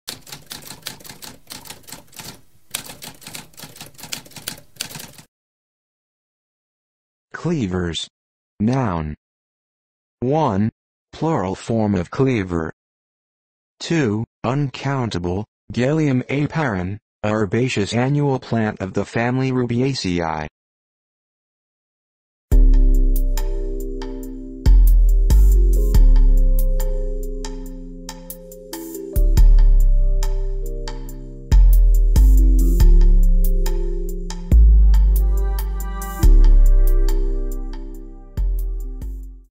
What does cleavers mean?
A spoken definition of cleavers.

Intro Sound:
Typewriter - Tamskp